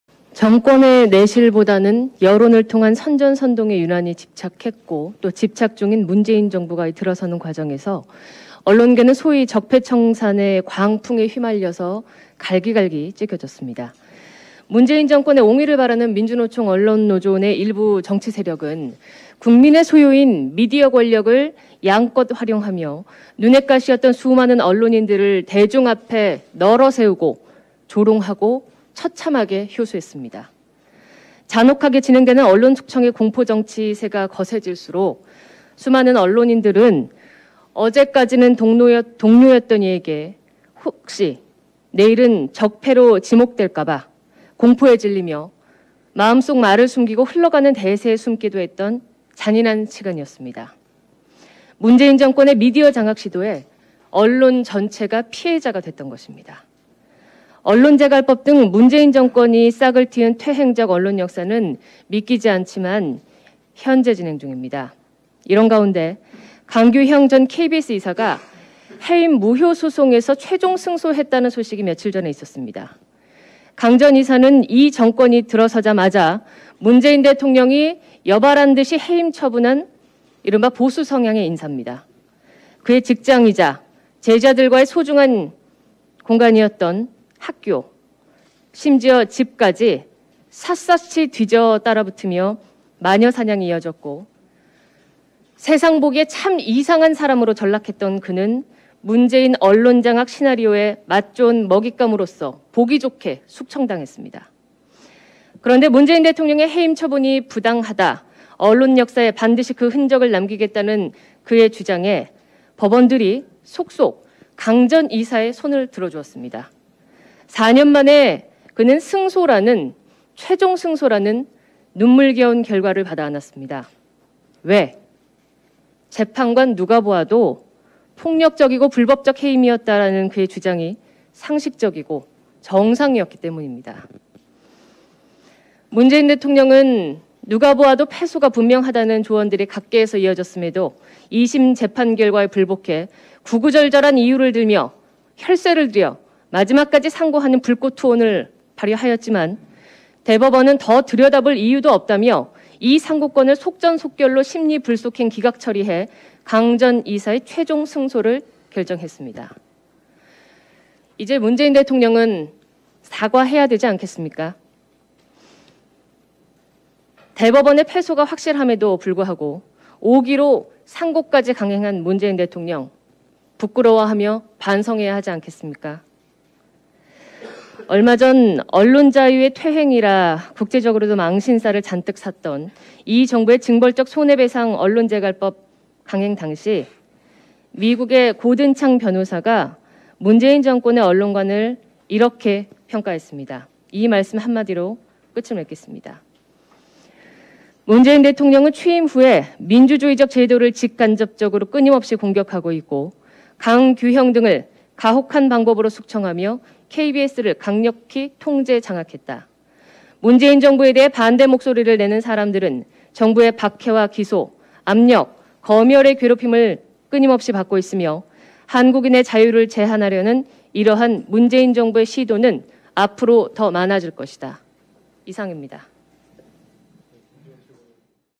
배현진 "정부 미디어 장악 시도에 언론 전체 피해자"
배현진 국민의힘 의원이 16일 국회에서 열린 최고위원회의에서 정부의 미디어 장악 시도에 관련해 발언하고 있다.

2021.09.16 일요서울TV 김정오 기자 *영상출처 - 국민의힘